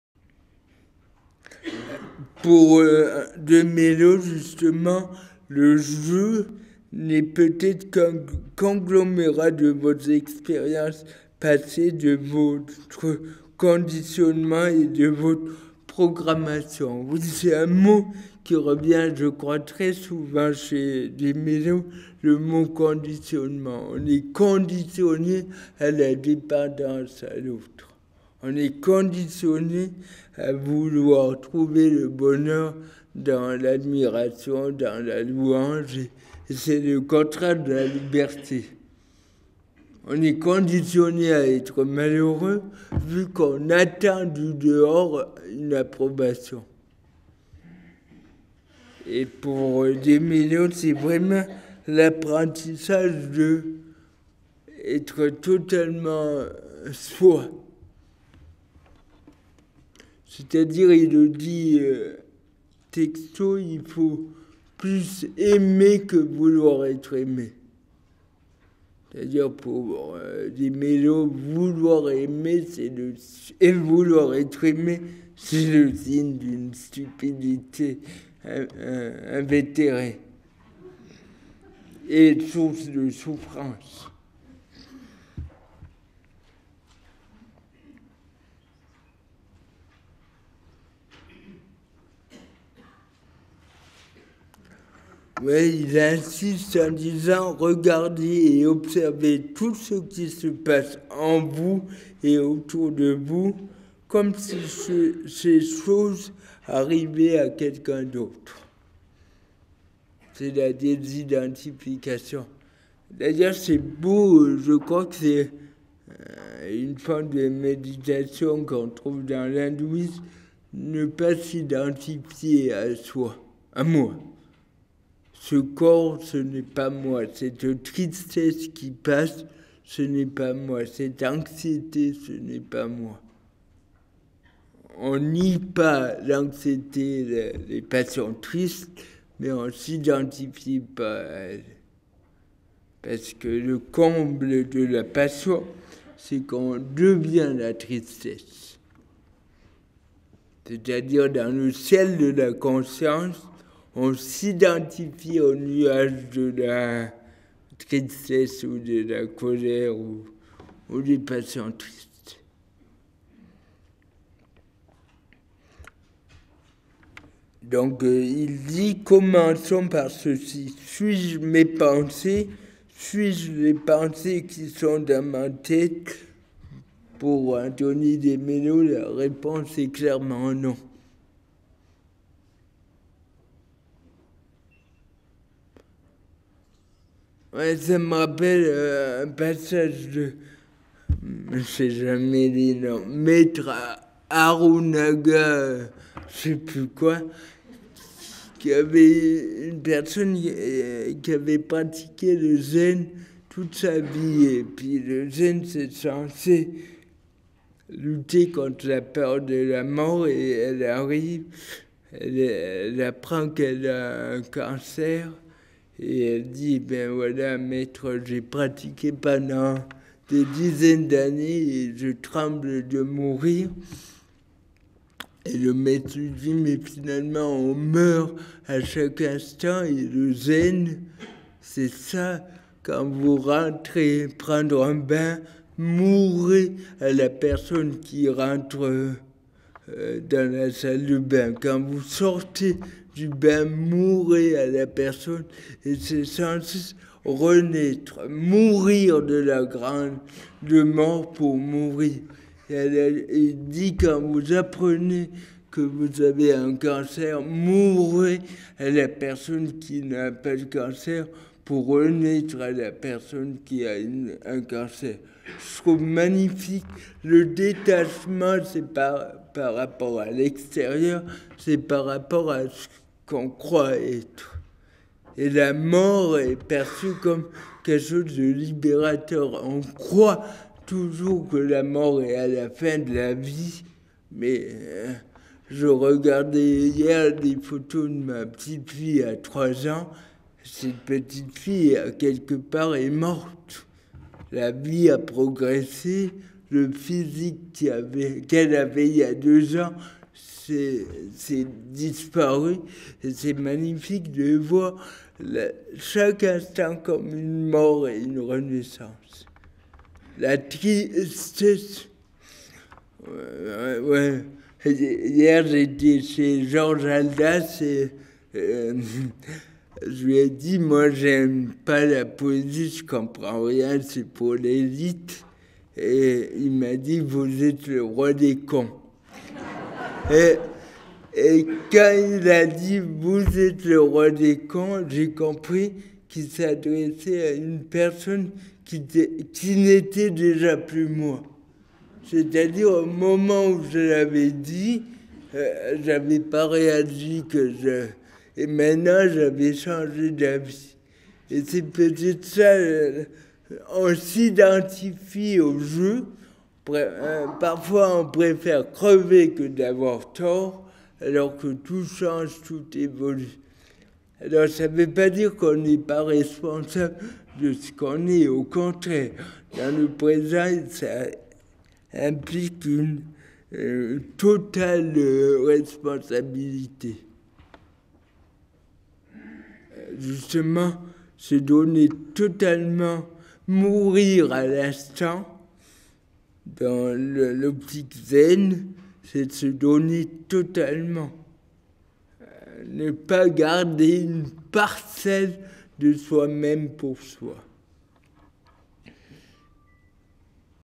Anthony de Mello, ou quand les étiquettes tombent
Le laboratoire philosophique des HUG. Extrait de la conférence d'Alexandre Jollien du 11 mars 2010
Alexandre Jollien se nourrit des philosophes et cite les textes avec une interprétation possible de leur pensée.

Pour De Mello, vouloir être aimé c'est le signe d'une stupidité invétérée et une source de souffrance.

"On croit toujours que la mort est à la fin de la vie, mais je regardais hier des photos de ma petite fille à trois ans, cette petite fille quelque part est morte, la vie a progressé, le physique qu'elle avait il y a deux ans a disparu. C'est magnifique de voir chaque instant comme une mort et une renaissance. "

Hier j'étais chez George Haldas et je lui ai dit: « Moi j'aime pas la poésie, je ne comprends rien, c'est pour l'élite ». Il m'a dit: « Vous êtes le roi des cons ». Quand il a dit « Vous êtes le roi des cons », j'ai compris qu'il s'adressait à une personne qui n'était déjà plus moi. C'est-à-dire au moment où je l'avais dit, j'avais changé d'avis. On s'identifie au « je », parfois on préfère crever que d'avoir tort alors que tout change et tout évolue.

A. Jollien

Conférence organisé par le Pr Alain Golay
Service d'enseignement thérapeutique pour maladies chroniques